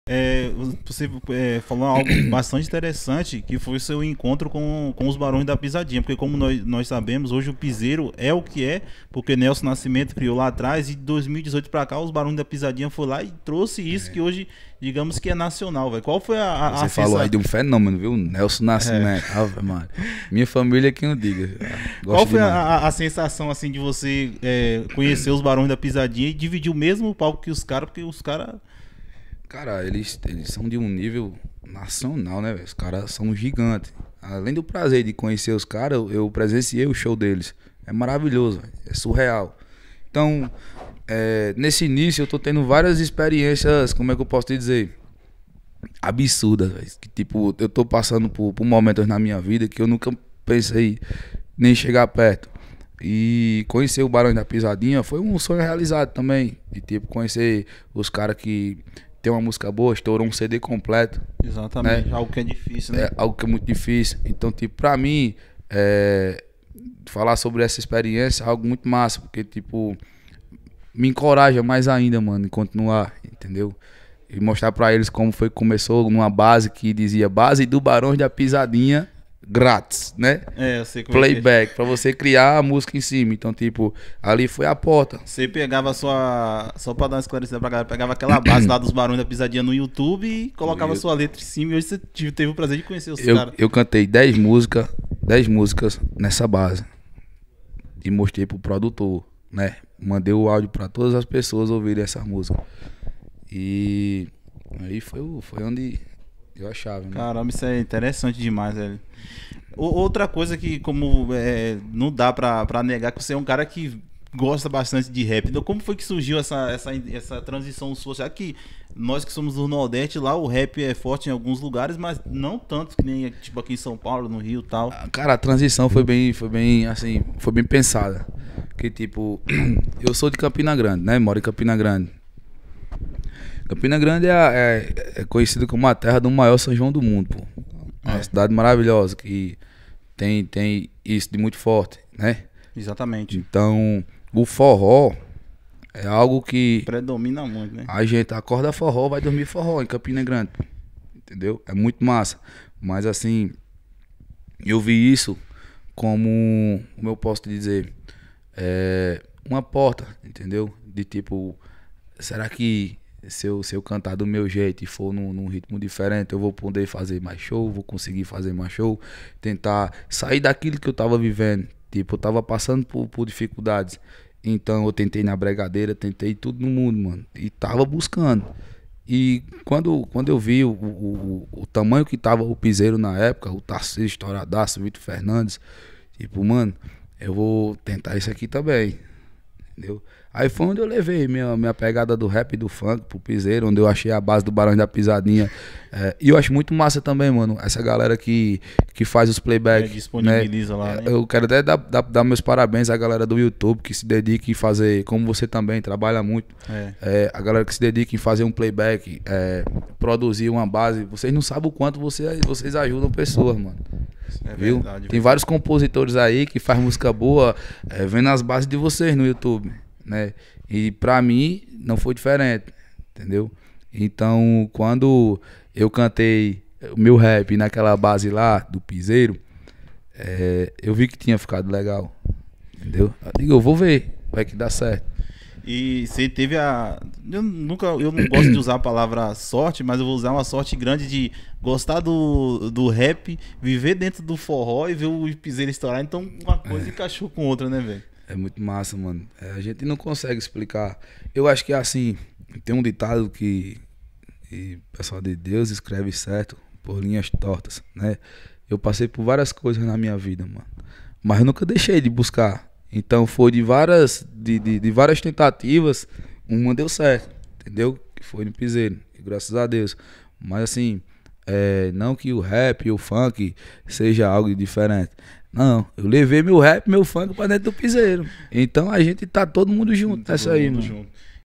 ENTENDA A IMPORTANCIA DE OS BARÕES DA PISADINHA NA VIDA DE JAPÃOZIN
Japãozin da Cachoeira” ou apenas Japãozinho, é cantor e compositor de 27 anos. O artista nasceu no interior de São Paulo, em Embu-Guaçu, mas, foi criado na Zona Leste de Campina Grande, no antigo Bairro da Cachoeira, interior da Paraíba, o que motivou o seu apelido e nome artístico.
Com 4 anos de carreira, o artista saiu do Rap, onde iniciou sua trajetória e viu no forró eletrônico uma oportunidade para lançar suas próprias músicas. O plano deu tão certo que, recentemente, Japãozin assinou com o Sua Música Records, Selo da plataforma Sua Música que atua na distribuição de fonogramas e no agenciamento de artistas.